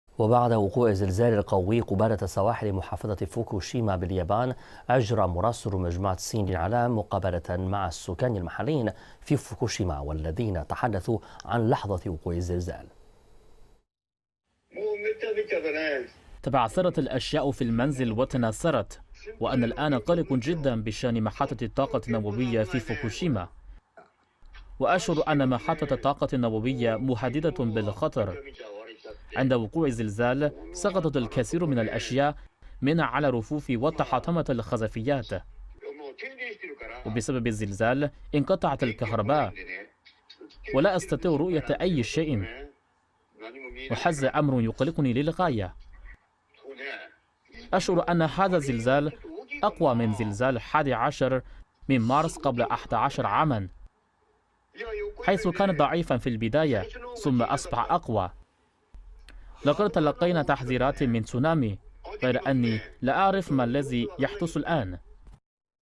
سكان فوكوشيما يتحدثون عن لحظة الزلزال
بعد وقوع الزلزال القوي قبالة سواحل محافظة فوكوشيما باليابان، أجرى مراسل مجموعة الصين للإعلام مقابلة مع السكان المحليين في فوكوشيما، والذين تحدثوا عن لحظة وقوع الزلزال.